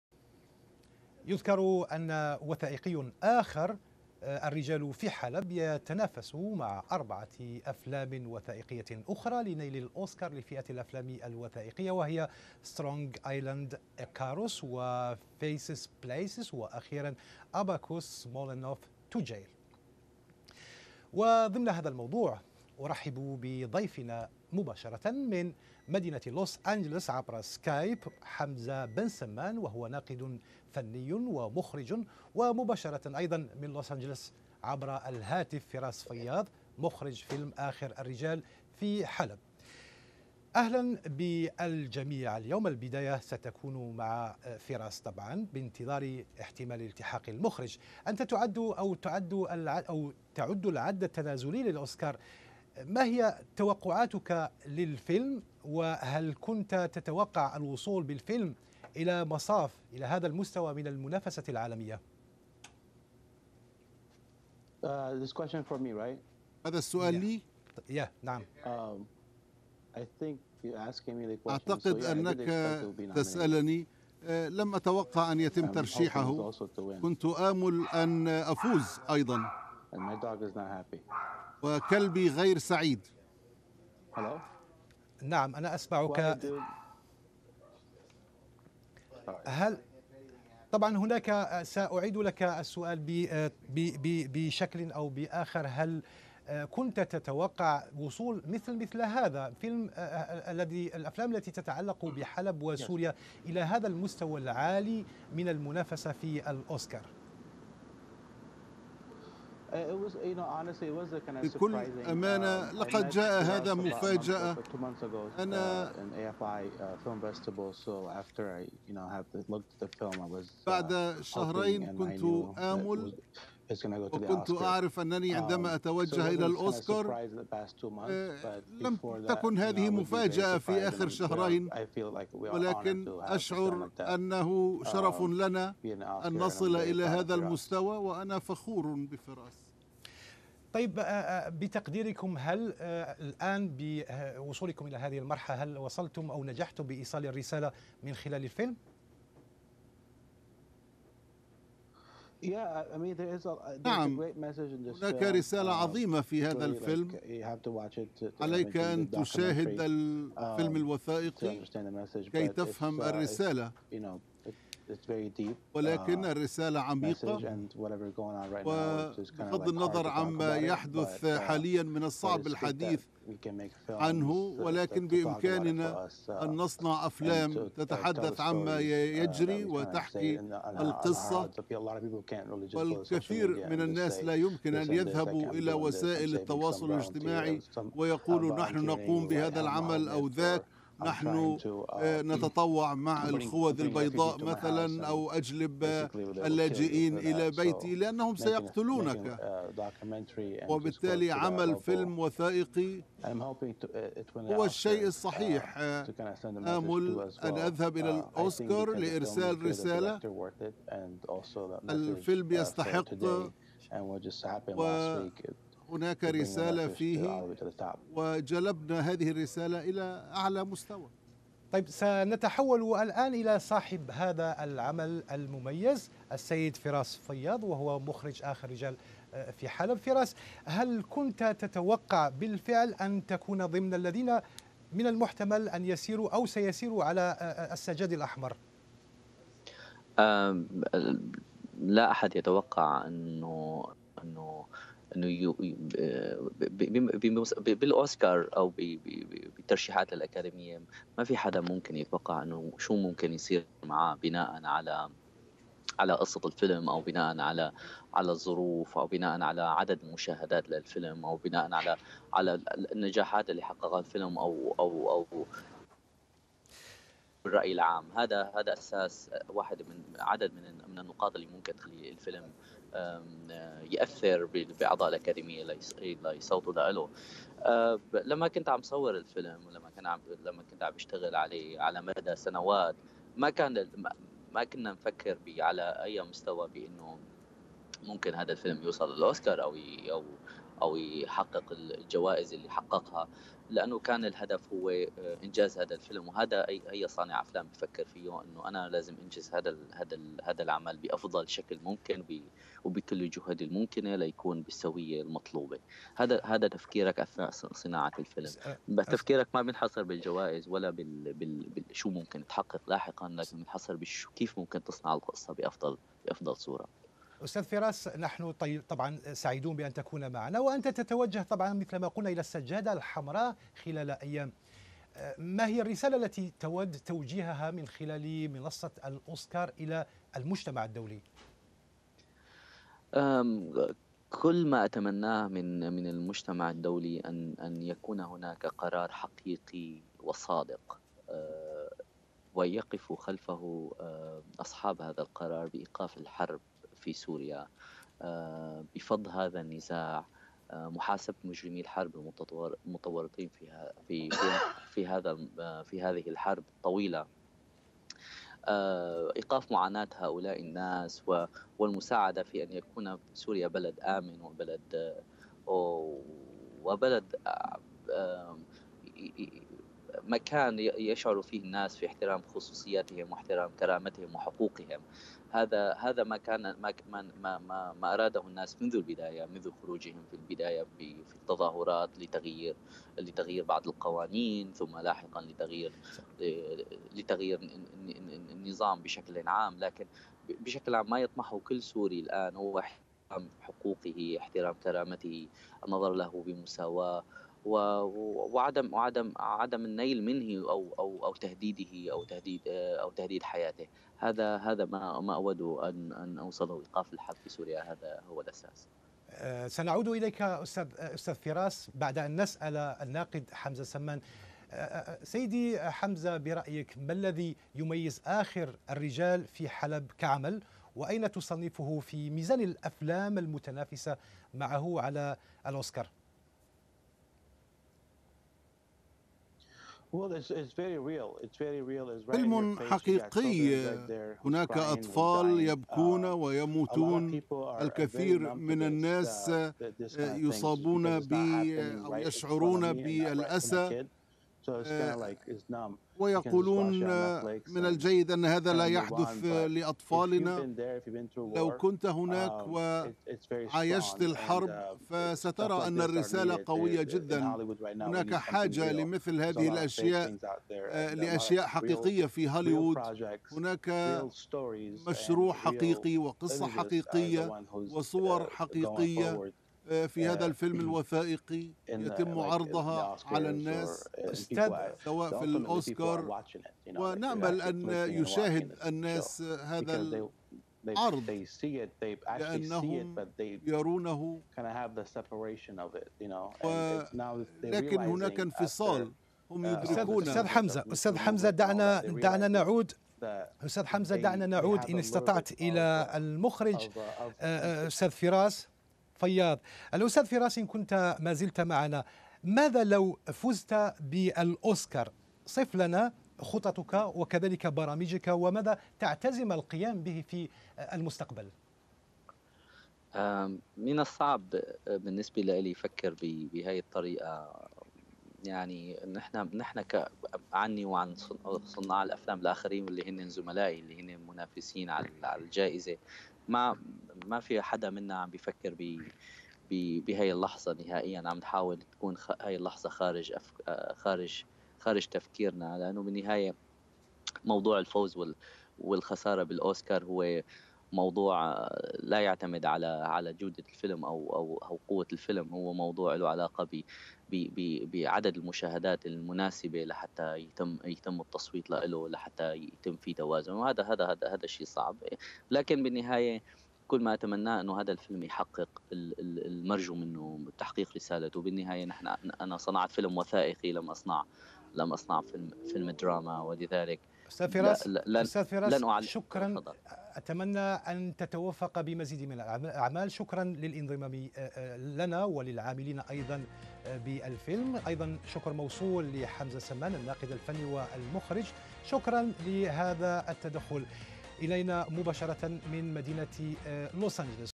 طريق " أخر الرجال في حلب" للأوسكار مع فراس فياض
آخر الرجال في حلب .. أول فيلم وثائقي سوري يرشح للأوسكار ، ومعه تصل رسالة واقع مرير يعيشه الأبرياء تحت حطام غارات وصواريخ الأسد وحلفائه. حوار مع حمزة سمان، المخرج والناقد الفني و فراس فياض مخرج فيلم آخر الرجال في حلب